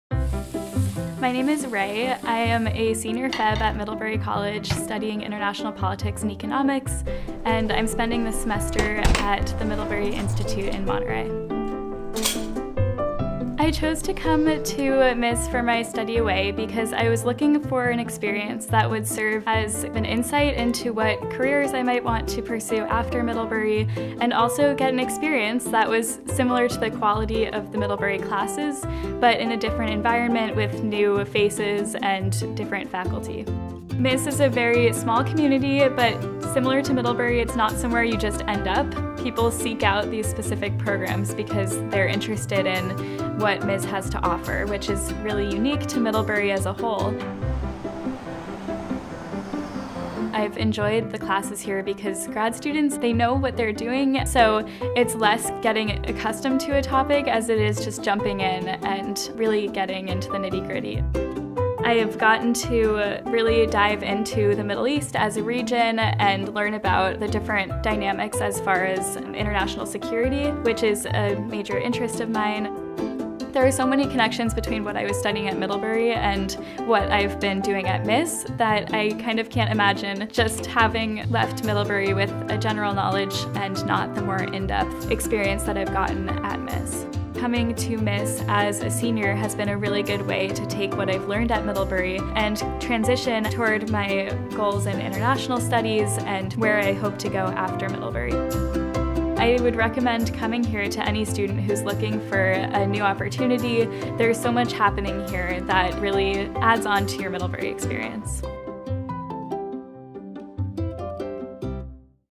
Middlebury - 2019 - Get a Head Start on What's Next
With the Study Away program, you can spend a semester or a year at the Middlebury Institute of International Studies—before you graduate from Middlebury College. Take graduate-level course in your area of interest and get a head start on a master’s degree. Work with expert faculty and field professionals to gain practical experience and skills at one of the world’s finest institutes of international studies.